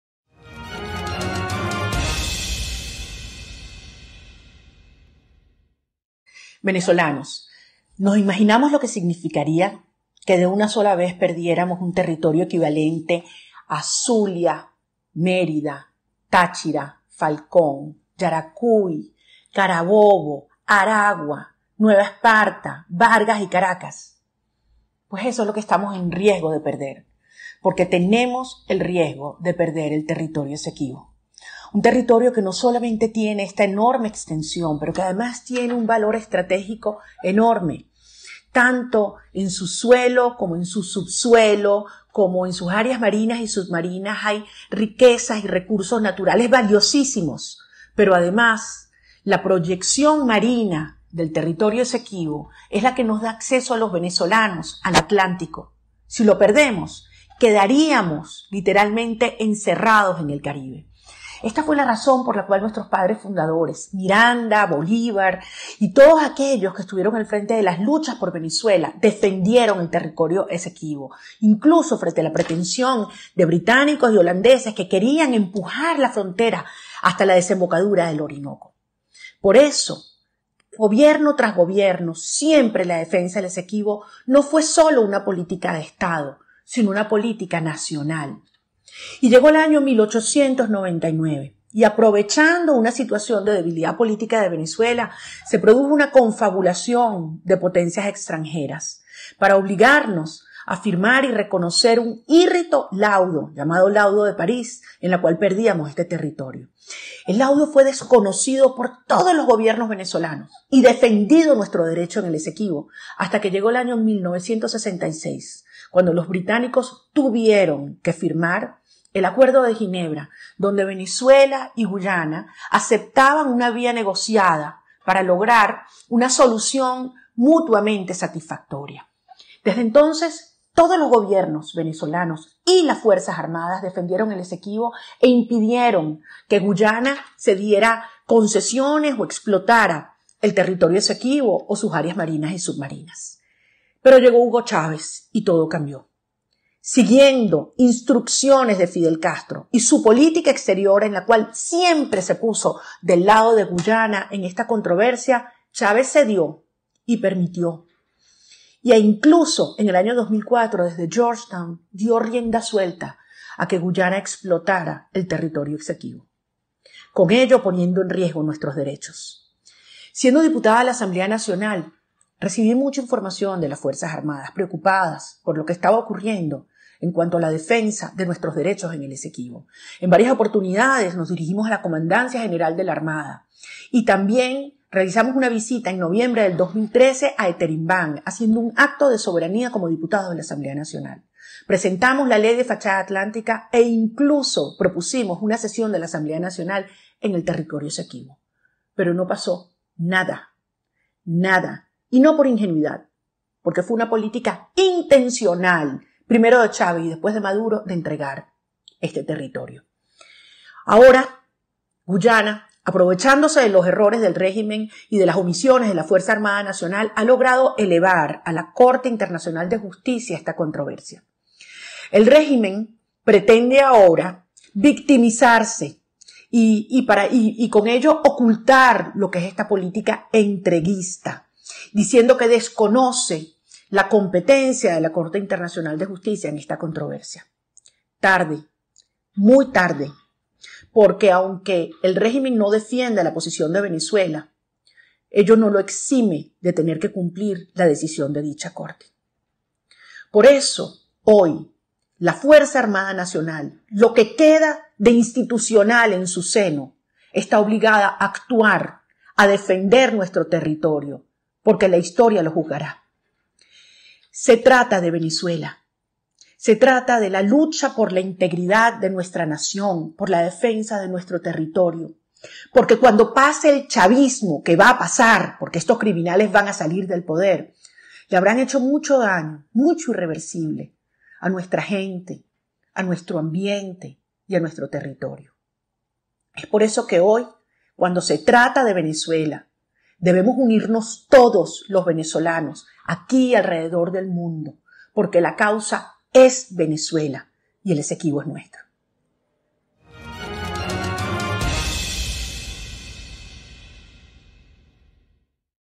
IMPORTANTE MENSAJE MARIA CORINA MACHADO A TODO EL PAÍS Y AL MUNDO
No claudicaremos en la defensa de nuestros derechos y nuestro territorio.